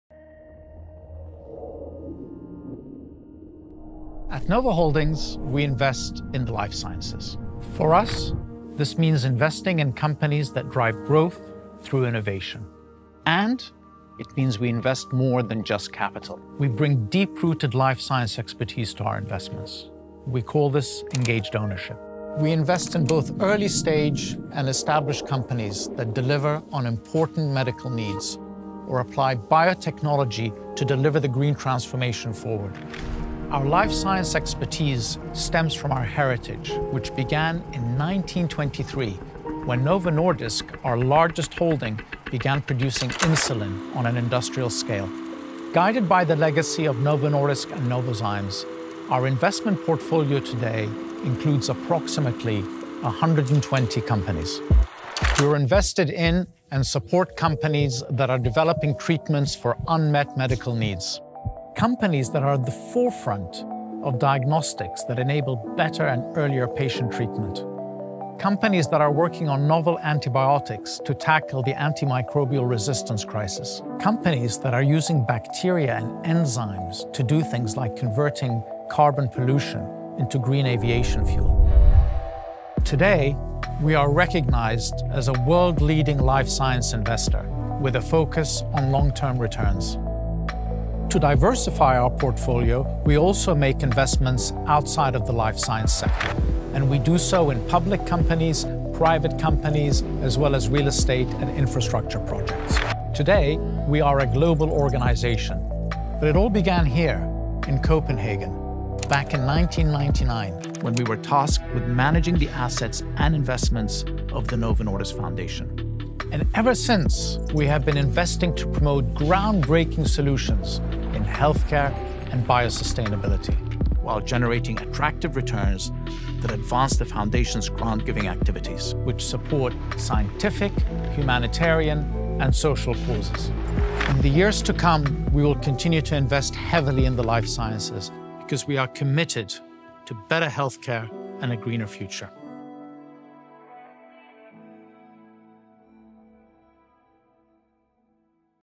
About Novo Holdings
Headquartered in Copenhagen, with a presence in San Francisco, Boston and Singapore, Novo Holdings is a world-leading life science investor with a focus on creating long-term value

Our mission is to be a supportive owner of the Novo Group companies and to make a growing and positive impact on health, science and society by generating attractive long-term returns on the assets of the Novo Nordisk Foundation.

"We bring more than capital to life science companies in all stages of development" - Kasim Kutay, CEO, Novo Holdings